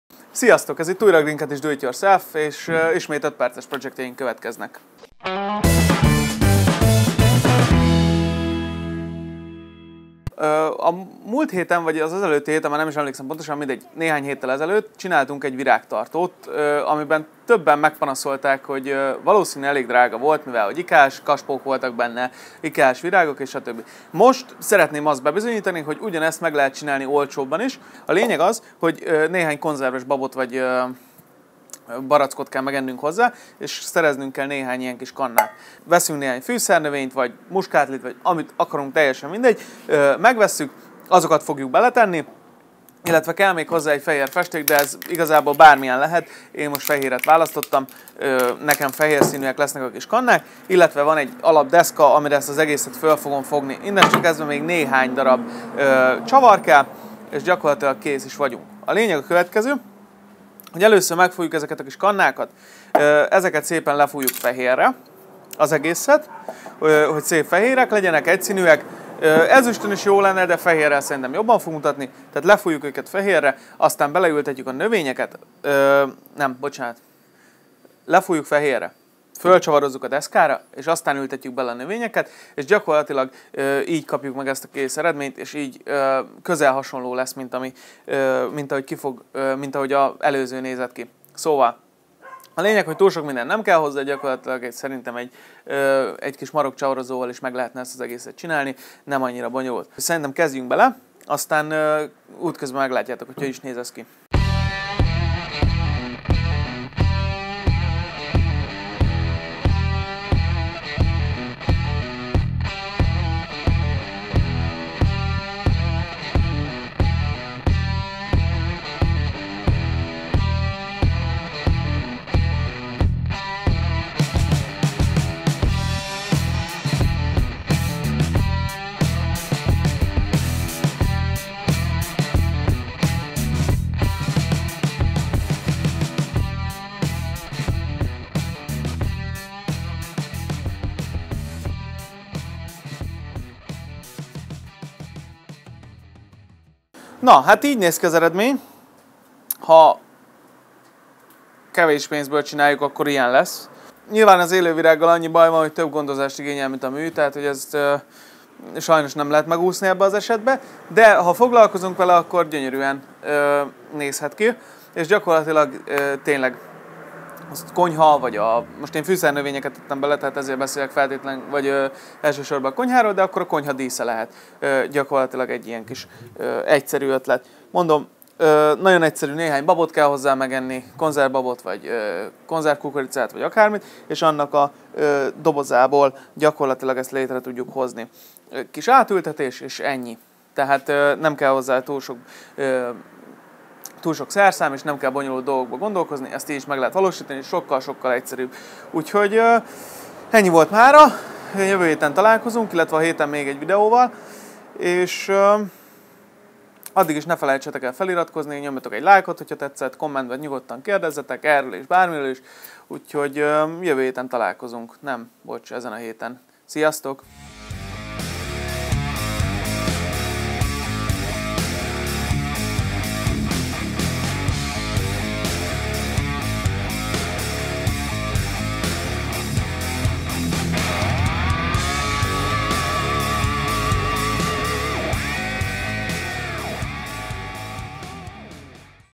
Decor a konyhába kevés pénzből | Green Cottage DIY - 5 PERCES TIPPEK
A legutóbbi videónkban amiben egy fali virágtartót készítettünk többen mondtátok, hogy a benne lévő virágok és kaspók eléggé megdrágítják a történetet. Hát most itt egy olcsóbb változat...